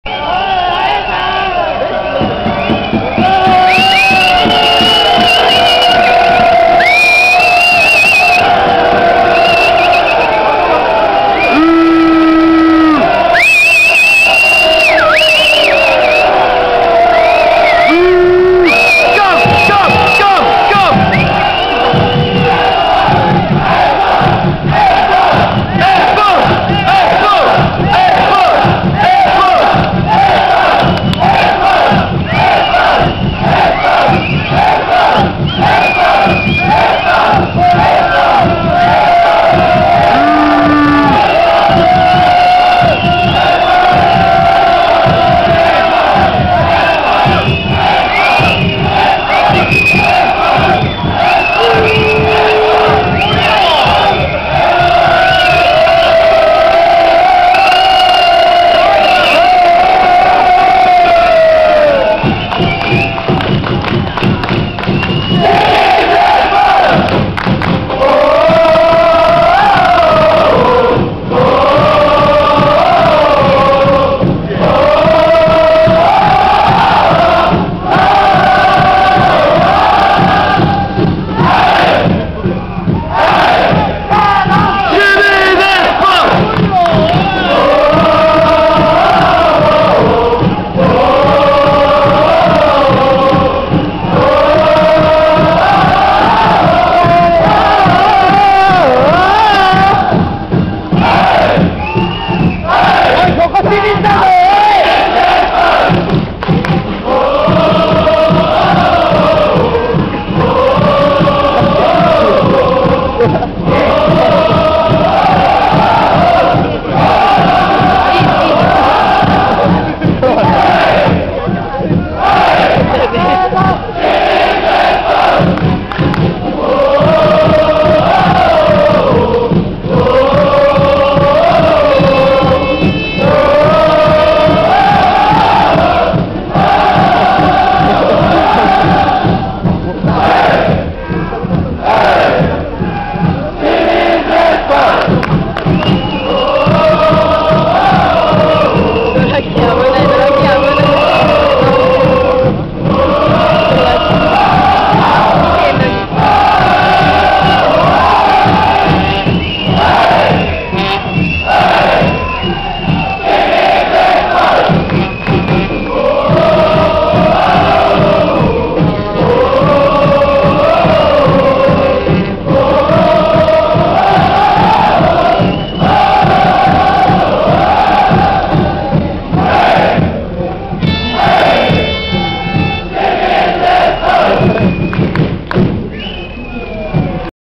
08静岡ダービー ジュビロ選手バス日本平入り Jubilo Iwata Team Bus
Jubilo team bus arriving at Nihondaira Stadium for the the Shizuoka Derby, 2008. Saturday May 3rd 2008.